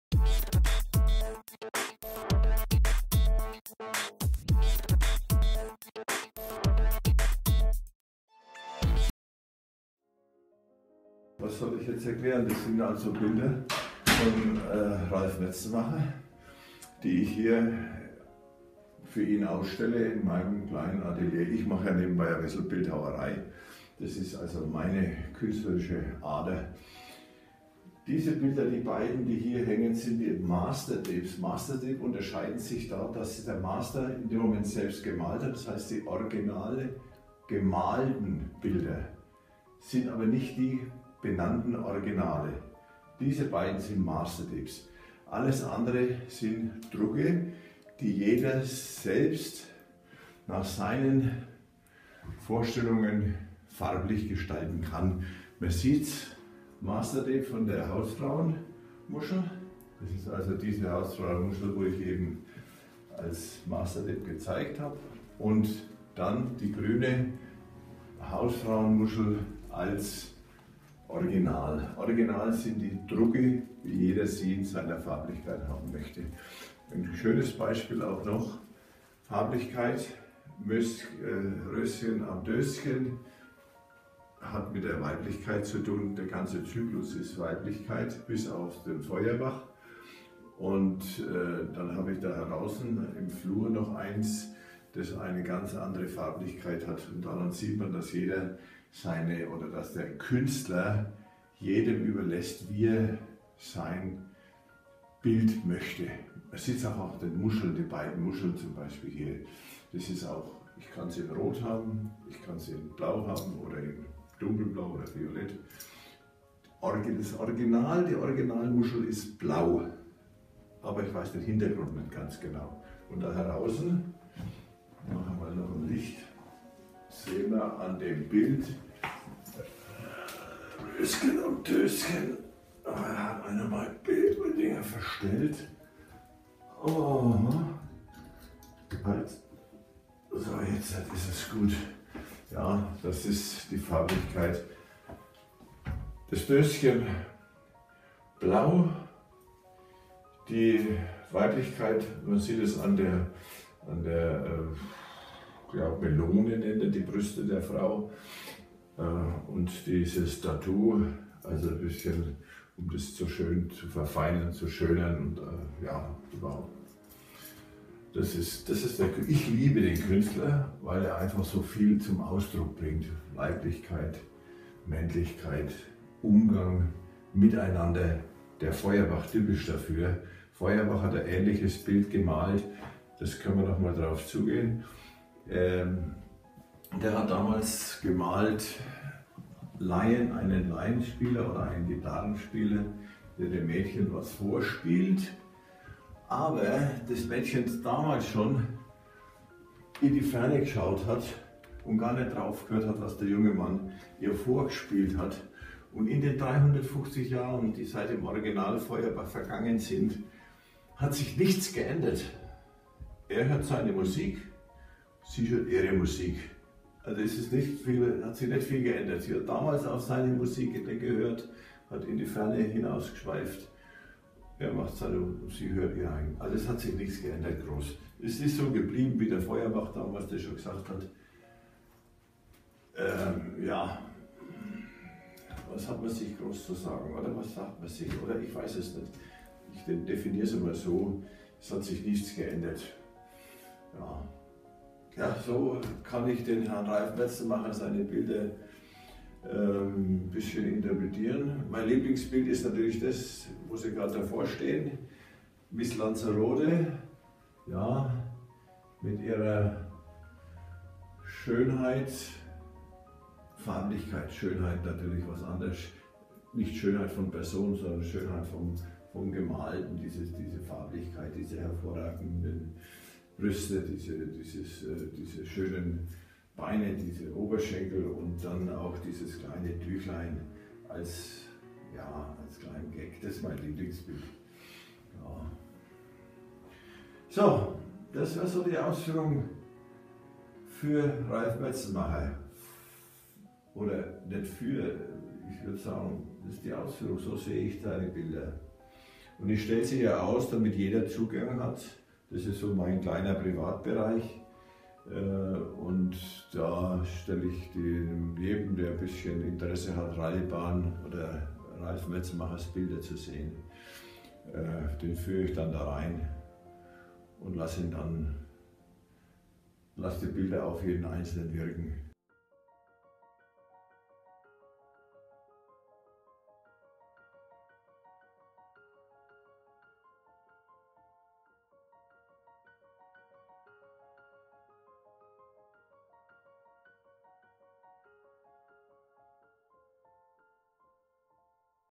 Hannes Bauer - Kunstfreund der Retro Art in seinen eigenen Worten
Hannes Bauer aus Bamberg - Kunstfreund der Retro Art in seinen eigenen Worten.